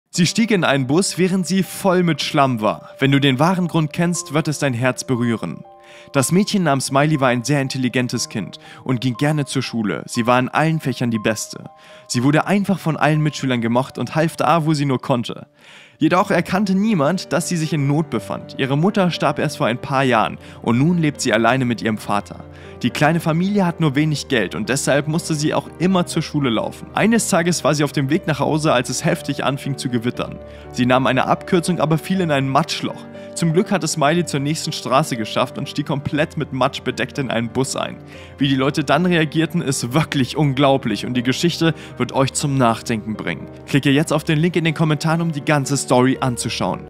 Voller Schlamm stieg sie in einen Bus - Der Wahre Grund wird dich Berühren!
Voller Schlamm stieg dieses Mädchen in einen Bus - Der Wahre Grund wird dich Berühren!

Miley war ein liebes Kind. In ihrer Schule war sie immer da, um jedem zu helfen, der Hilfe brauchte. Sie war sehr intelligent und gehörte in allen Fächern immer zu den Besten ihrer Klasse, aber das hielt sie nicht davon ab, ihre Mitschüler wie ihre Geschwister zu behandeln, ohne sich jemals überlegen zu fühlen, und die wiederum bewunderten sie.